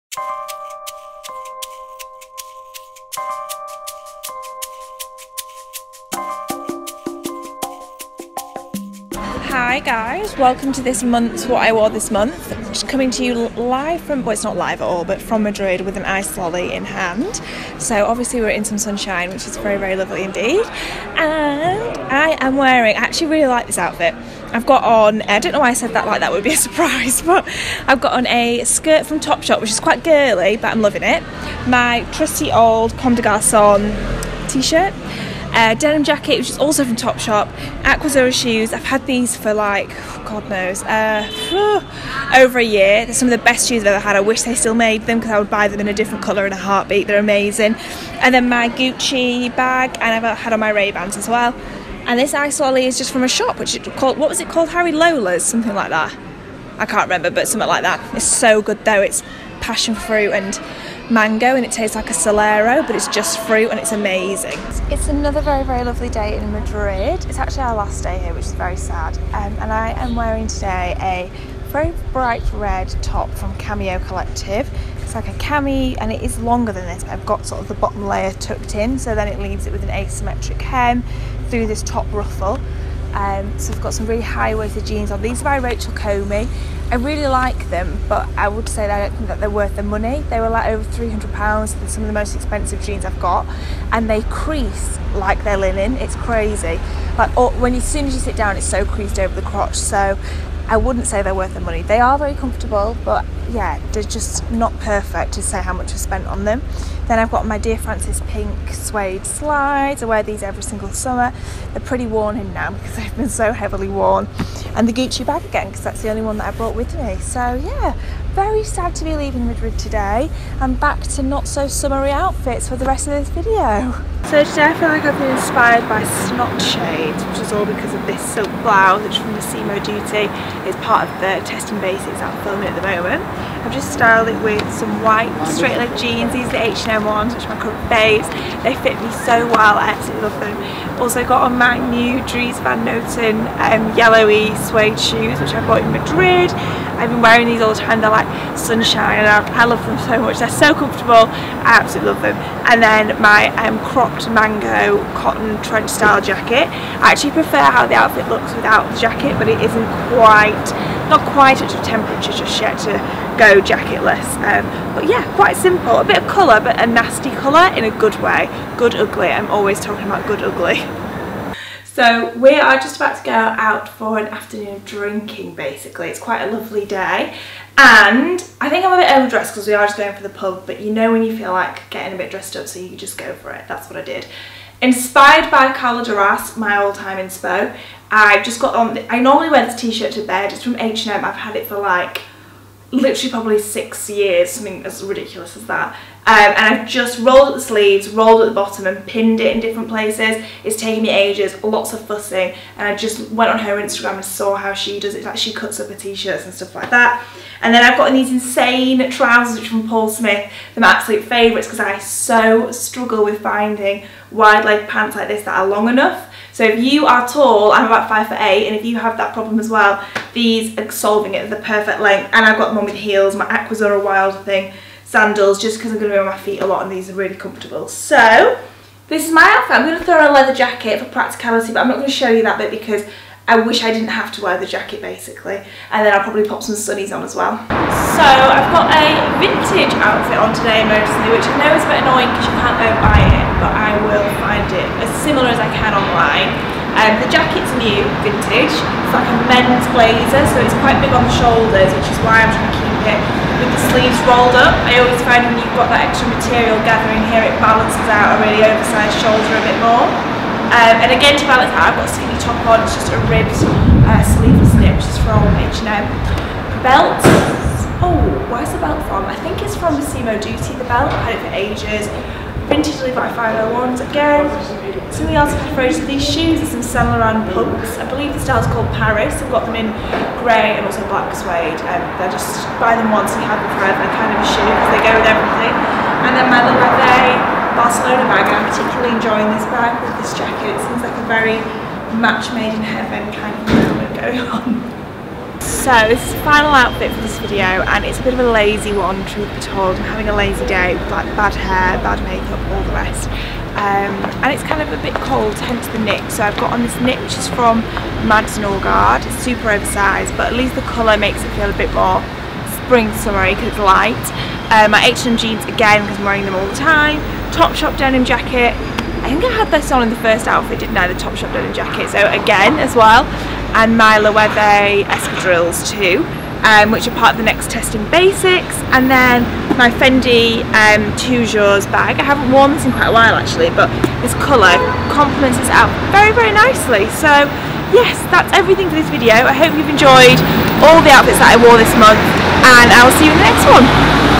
What I Wore This Month
Some Spring/Summer looks I have been wearing throughout April! Hope you all enjoy!

➤ 'lizzyhadfield'

OUTFIT 1:

TOPSHOP JACKET

TOPSHOP SKIRT

AQUAZZURA SHOES
➤old

COMME DES GARCON TOP

OUTFIT 3:

MASSIMO DUTTI SHIRT
➤ sold out

H&M JEANS

MANGO JACKET

H&M T-SHIRT

OUTFIT 5:

VINTAGE JACKET

VINTAGE LEVI'S 501S

SAINT LAURENT PUMPS
➤ old

OUTFIT 6:

MADS NORGAARD KNIT

H&M JEANS

TOPSHOP DENIM JACKET

FENDI BAG